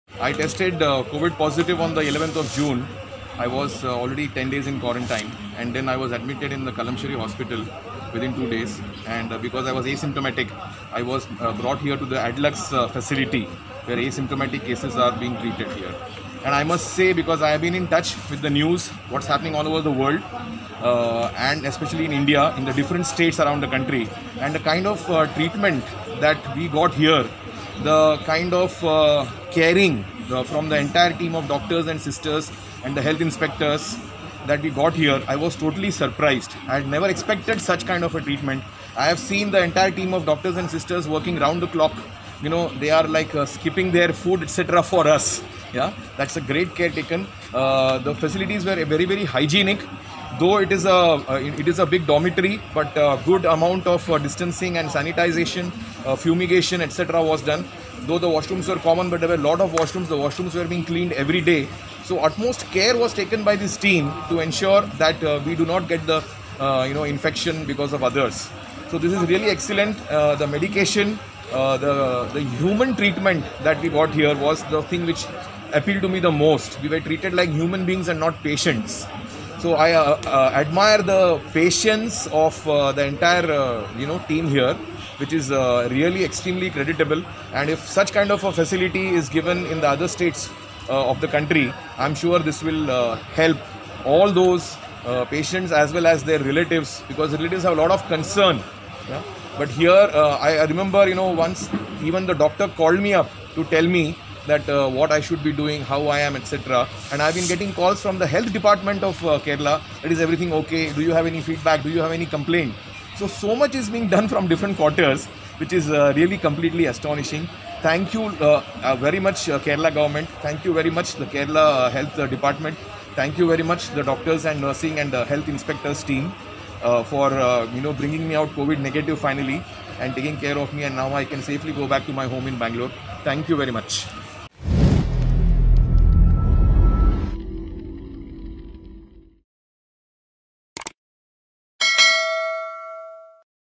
ശരിക്കും കേരളം ഞെട്ടിച്ചു, ഈ കരുതലിന് വാക്കുകളില്ല ; കോവിഡ് ഭേദമായ ആളുടെ വാക്കുകൾ | Covid19 | Kerala
ശരിക്കും കേരളം ഞെട്ടിച്ചു, ഈ കരുതലിന് വാക്കുകളില്ല ; കോവിഡ് ഭേദമായ ആളുടെ വാക്കുകൾ